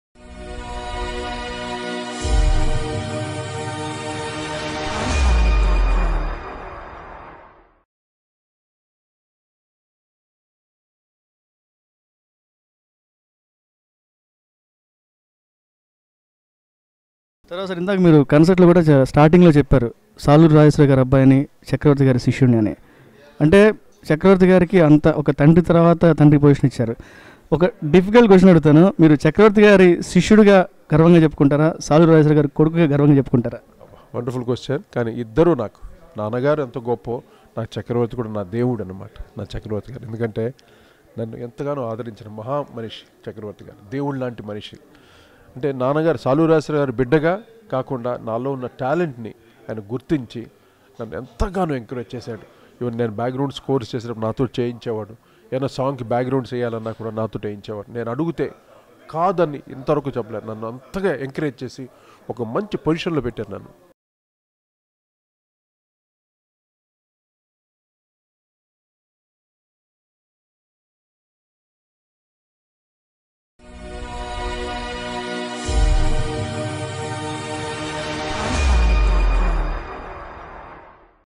"Chakravarthi garu is the reason for my position now" - Music Director Koti | Full Interview on DPTV
Telugu Association of North America celebrated 'Koti Sarigamalu' event at Hutto Performing Arts Center, Hutto, TX on 10th Feb, 2018.

Koti along with his team Hema Chandra, Shravana Bhargavi, Pruthvi Chandra, Ranina Reddy enteratined the Texas audience. Around 1000 people attended the event and everyone made the event grand success.